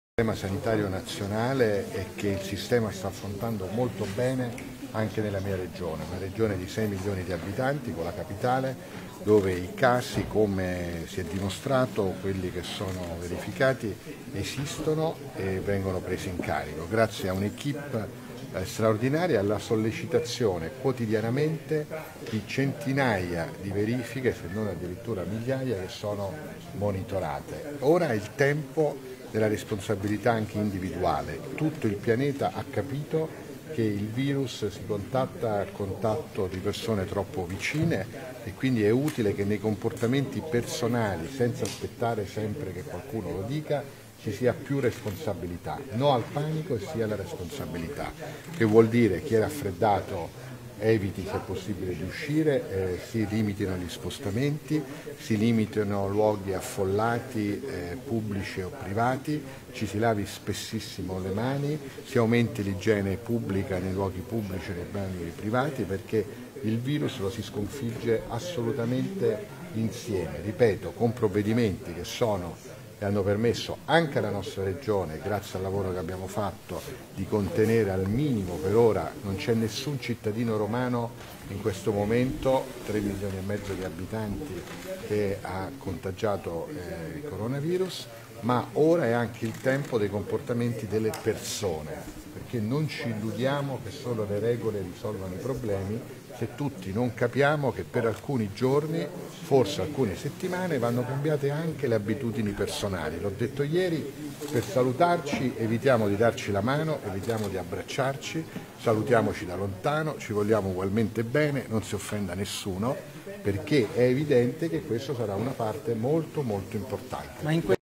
Zingaretti -E' importante che ognuno di noi faccia la propria parte (03.03.20)
- Zingaretti - Sull’emergenza Coronavirus, in attesa che vengano presi nuovi provvedimenti restrittivi che intervengano sugli eventi, sulle abitudini e sui comportamenti individuali, è importante che ognuno di noi faccia la propria parte per contenere la diffusione del virus seguendo le indicazioni della scienza, come ad esempio mantenere una distanza di sicurezza con le altre persone di almeno un metro per ridurre il rischio di contagio, lavandosi spesso le mani e non toccandosi occhi, naso e bocca, riducendo la partecipazione a eventi pubblici o affollati (03.03.20)